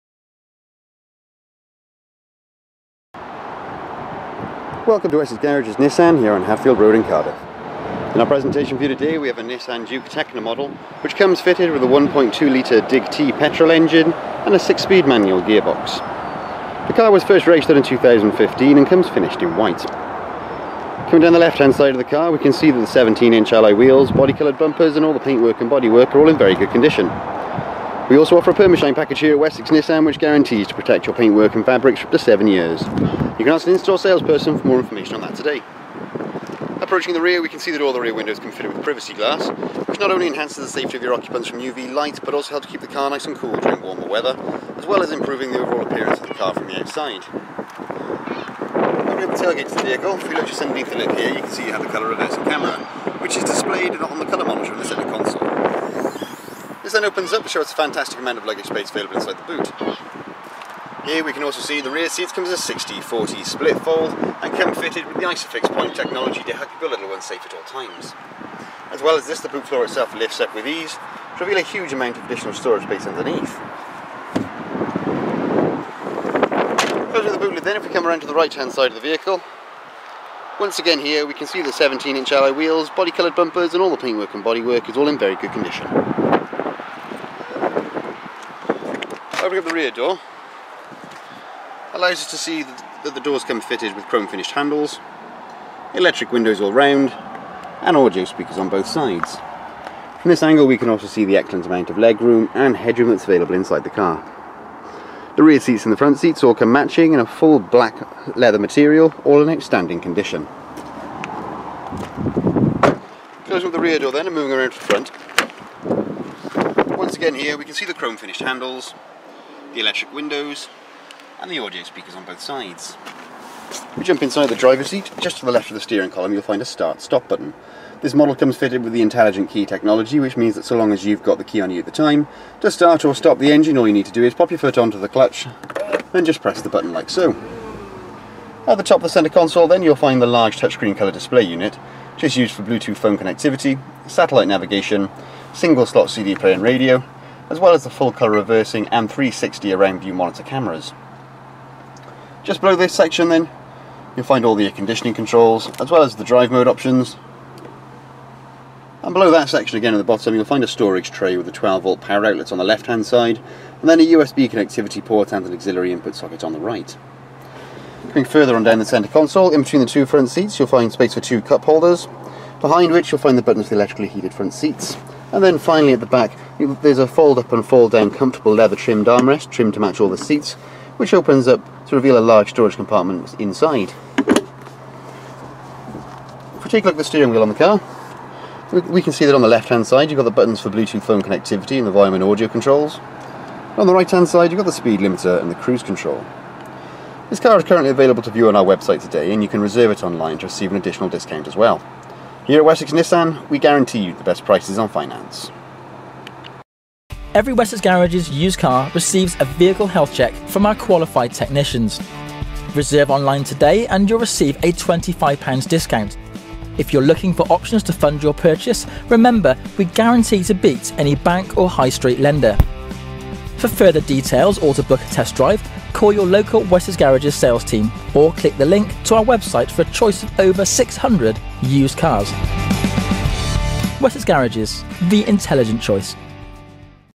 Wessex Garages | Nissan Juke Tekna DiG-T at Hadfield Road, Cardiff | VK65WVB
You are viewing the Nissan Juke Tekna 1.2 DiG-T Petrol model with a Manual gearbox, finished in White at our showroom on Hadfield Road in Cardiff.